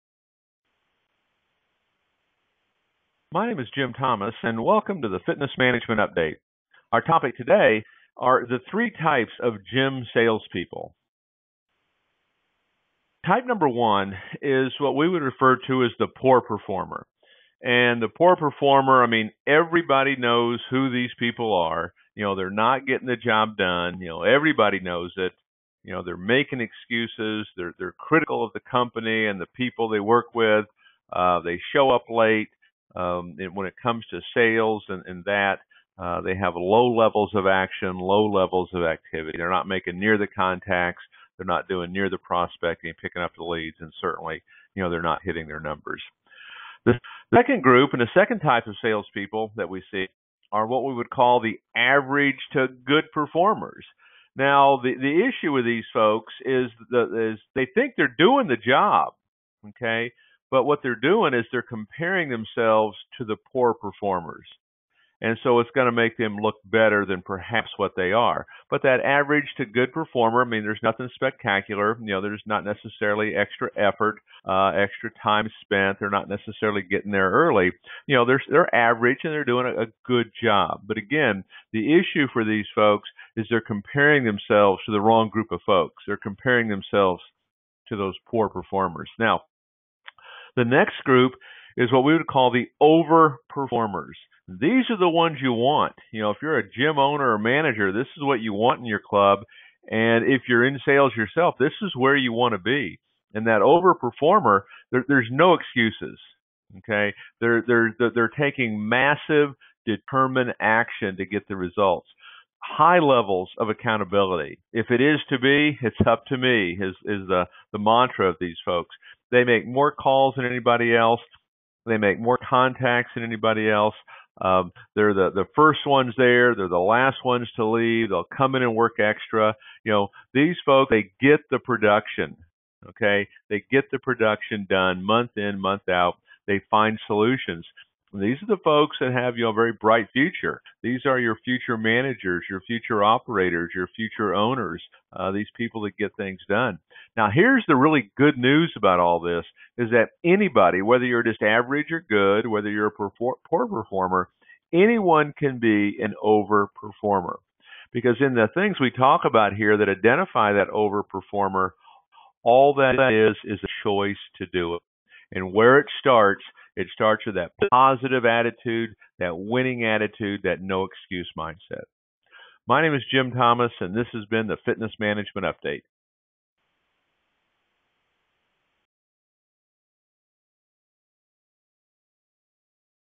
3 Types of Gym Salespeople
Jim Thomas of Fitness Management & Consulting discusses the 3 types of gym salespeople.